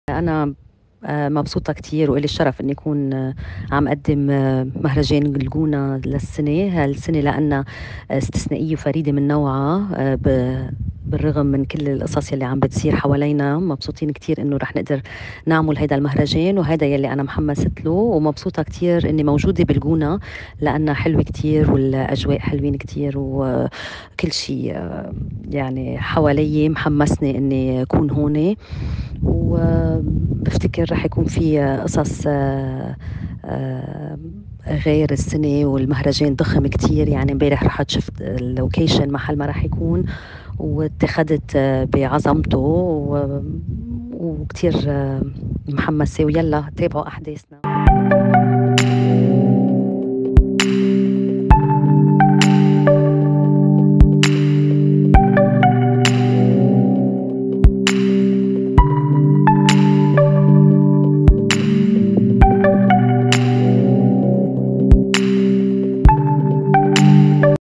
هيلدا خليفة تكشف للوطن حقيقة تقديمها حفل افتتاح الجونة
جريدة الوطن
جريدة يومية مستقلة